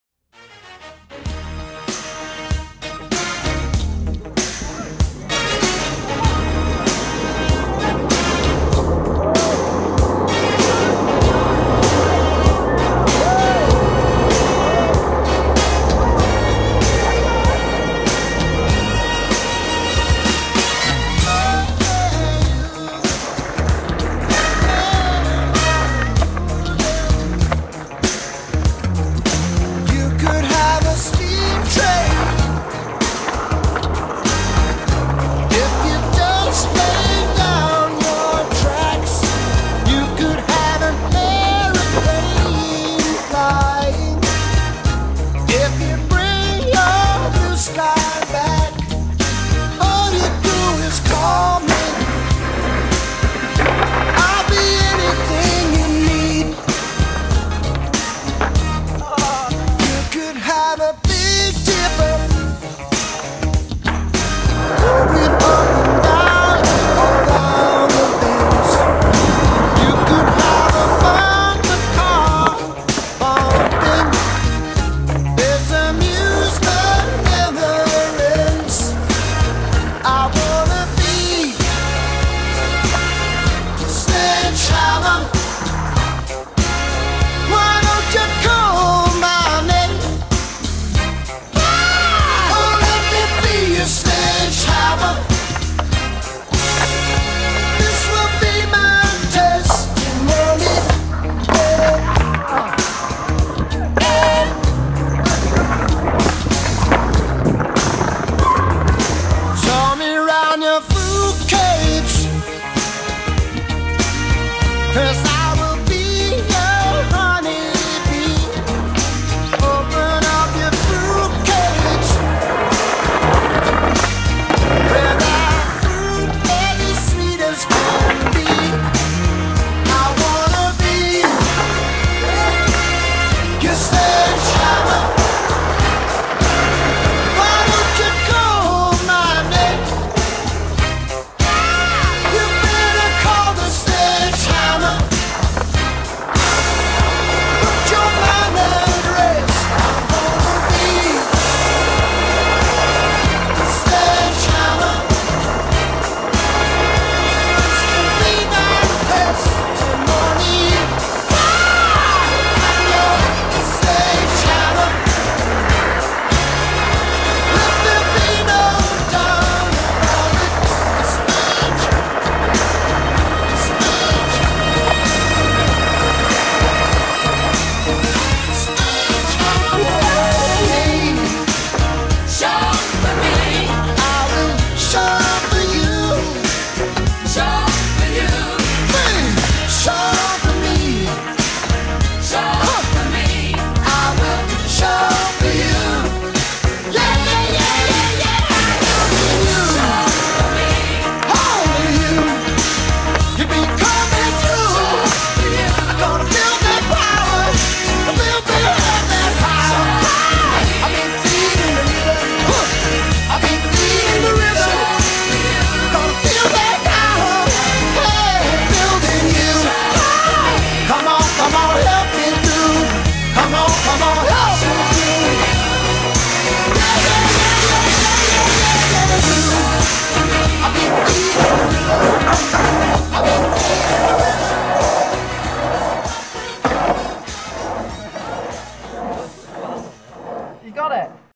HoWS August Montage
Skate montage filmed in llandrindod wells with the heart of wales skate team